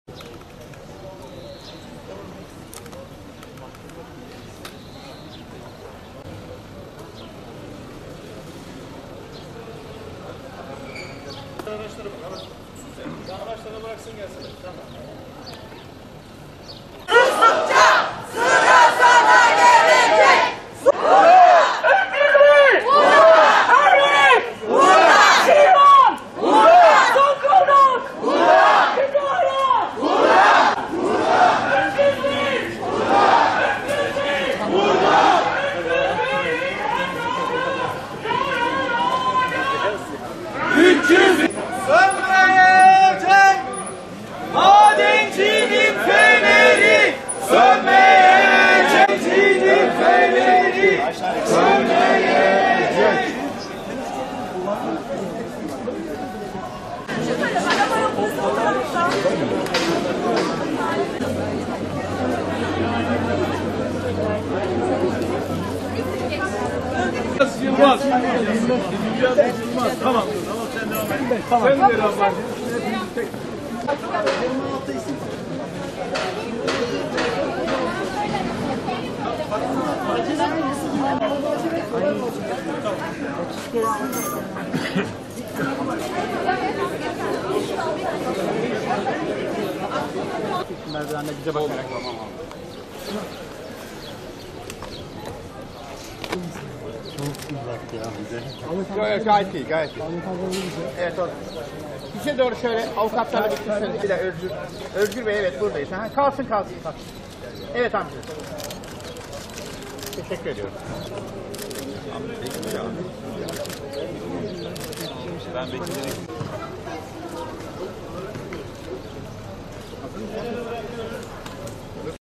Soma Davası karar duruşması o tarihe ertelendi
301 işçinin hayatını kaybettiği Soma maden kazasının karar davası mahkeme heyetinin yoklama almasının ardından 11 Temmuz Çarşamba Gününe ertelendi.